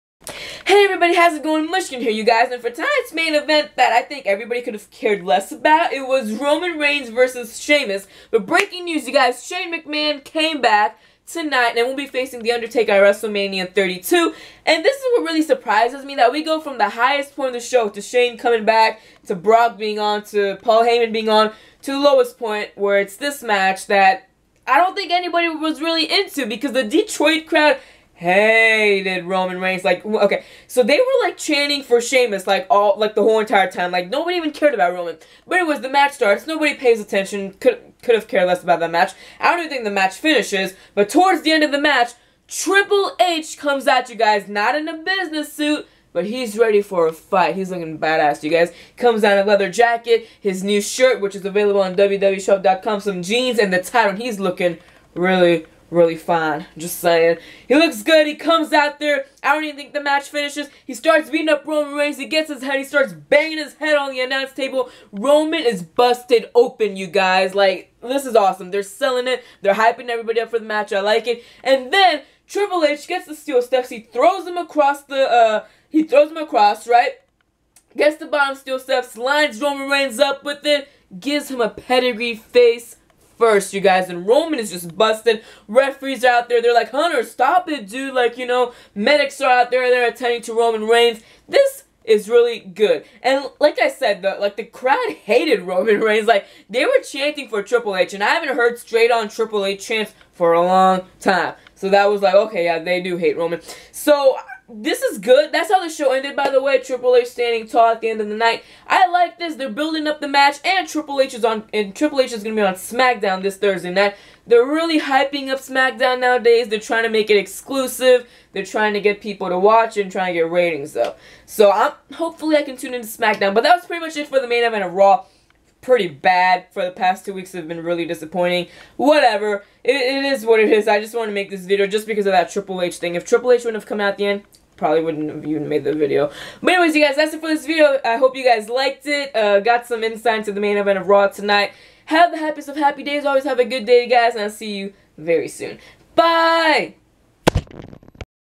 Triple H On WWE RAW 2/22/16 Review
My review and thoughts on Triple H being on RAW tonight !

Add me on SnapChat: MushkanJuma